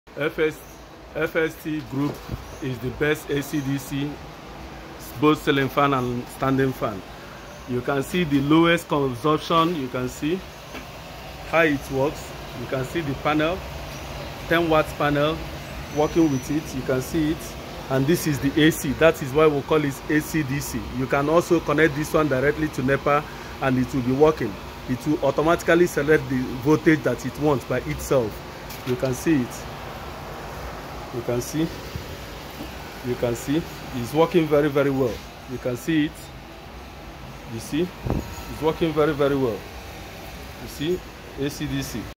FST Group Solar Fan
FST Group Solar Fan is an AC / DC with a very low consumption current and a cool air flow fan.